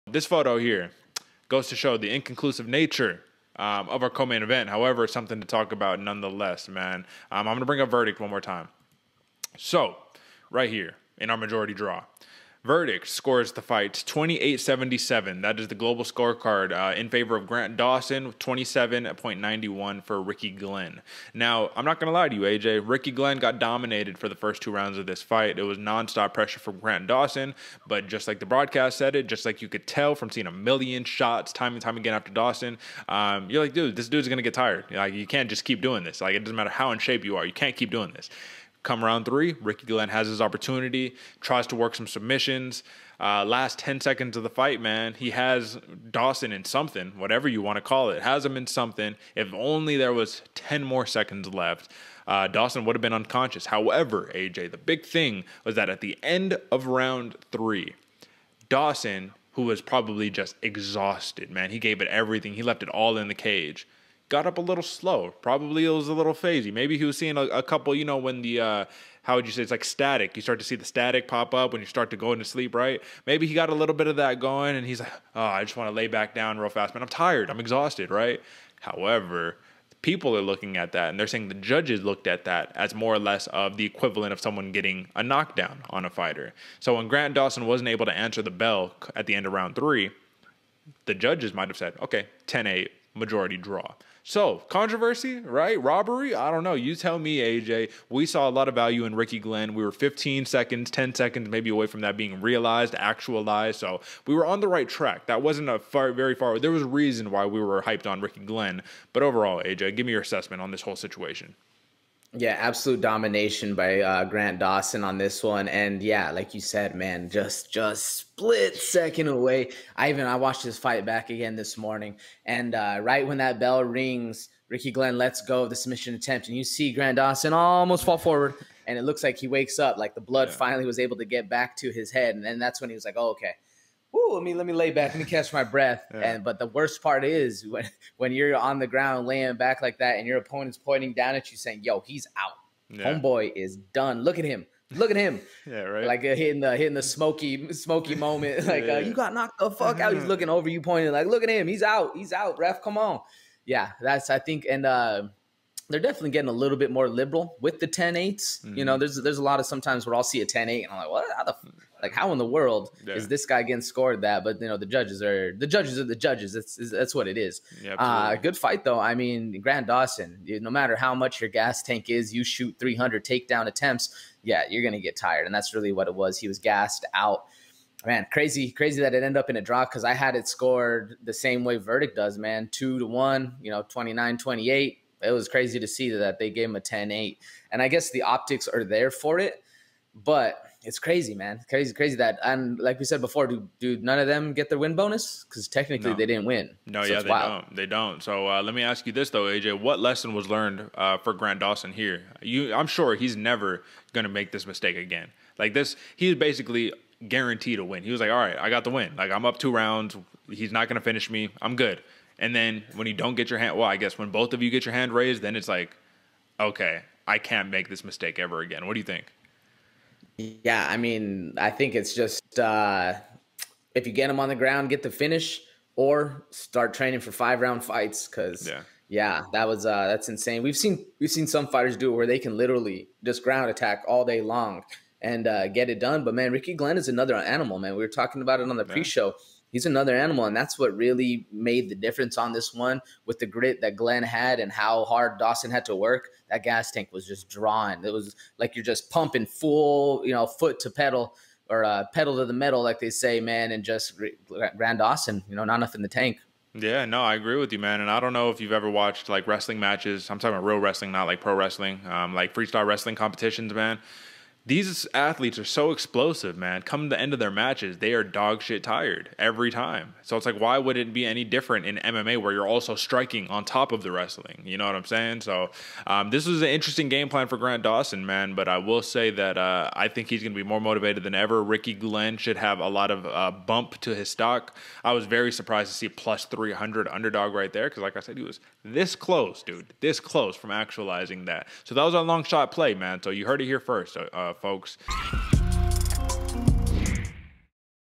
GRANT DAWSON and RICKY GLENN FIGHT TO A MAJORITY DRAW | UFCVegas41 | Bloody Water Podcast
This clip is taken from Bloody Water Podcast Ep. #90 | UFC Vegas 41 Post-Show